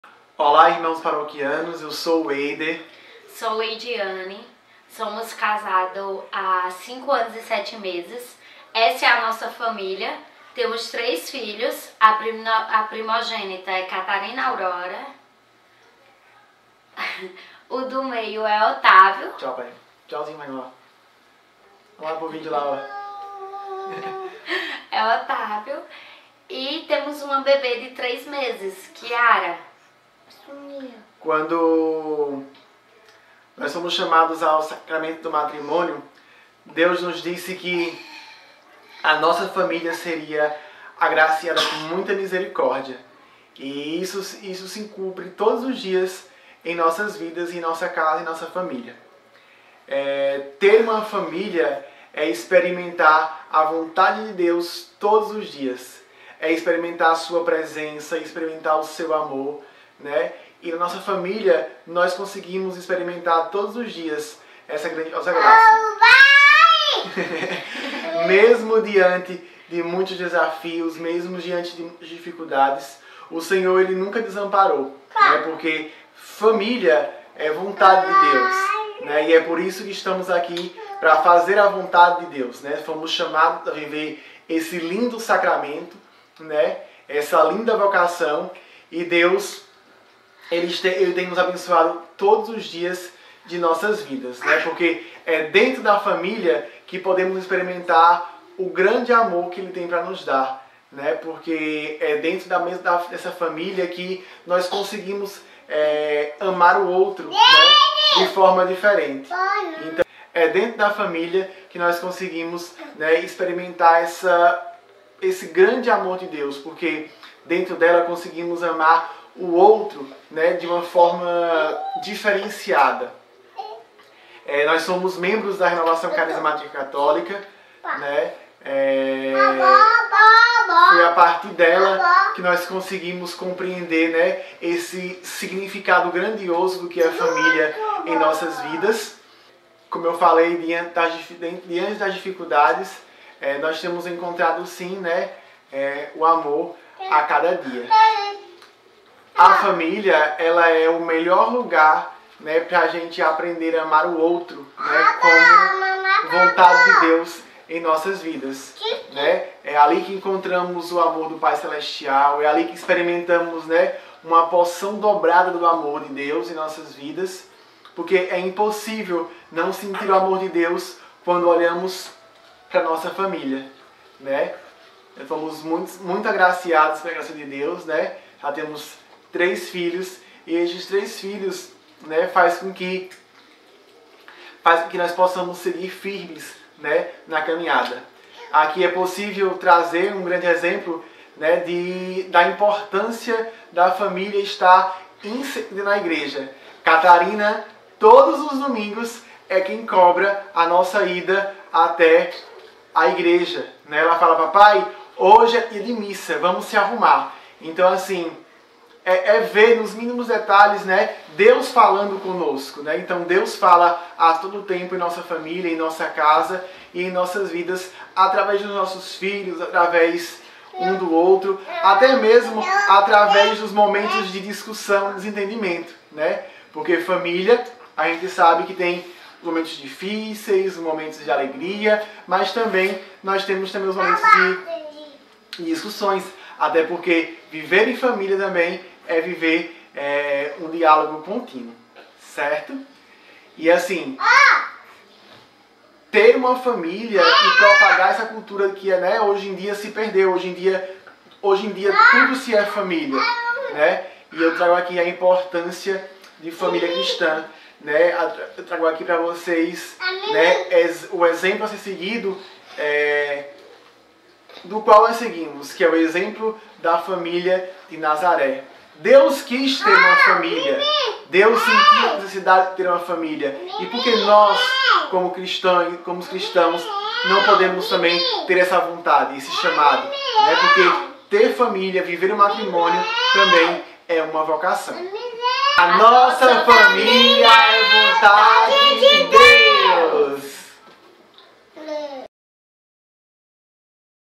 Vocação Família - Weider & Leidiane
Neste segundo domingo de agosto, rezamos pela vocação família (matrimonial), e convidamos a família de Weider e Leidiane para falar um pouco dessa vocação.